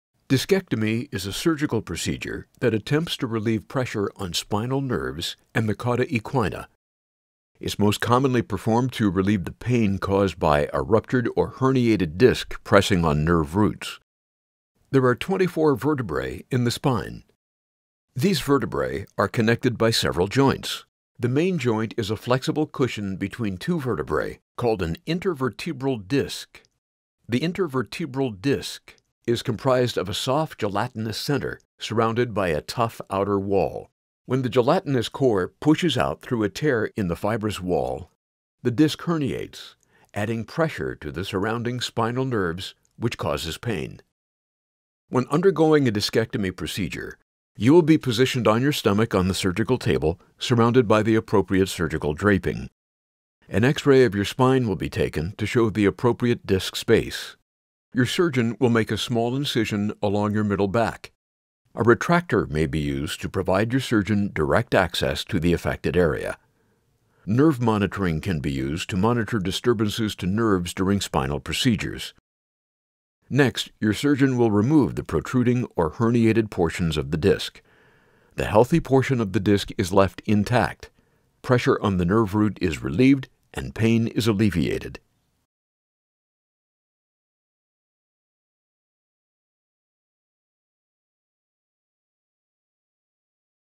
Discectomy Patient Education
Дискэктомия представляет собой хирургическое вмешательство, в ходе которого удаляется весь диск или только его выпирающая часть. Иногда она сочетается с ламинэктомией, подразумевающей удаление части дужки позвонка. Когда во время процедуры удаляется часть межпозвонкового диска (грыжа), сдавливающий нервный корешок.